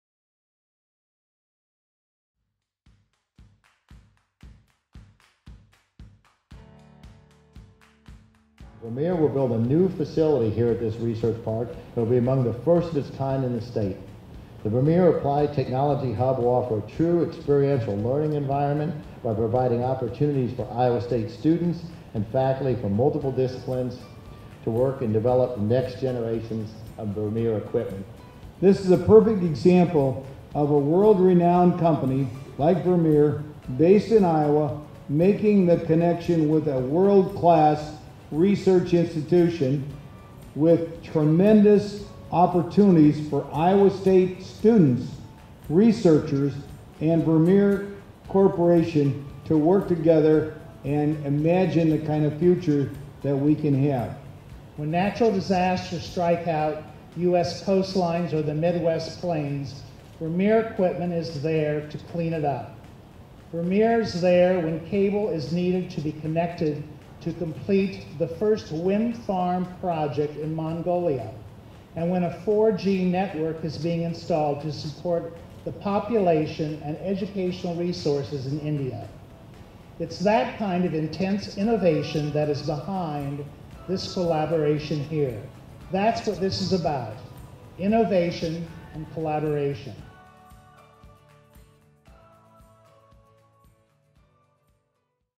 Vermeer Applied Technology Hub Breaks Ground
Vermeer Corporation celebrates the groundbreaking of their facility, the Vermeer Applied Technology Hub, in the Iowa State Research Park in Ames, Iowa. This facility will house opportunities for innovation and collaboration between Iowa State students and Vermeer team members as we develop the next generation of Vermeer equipment.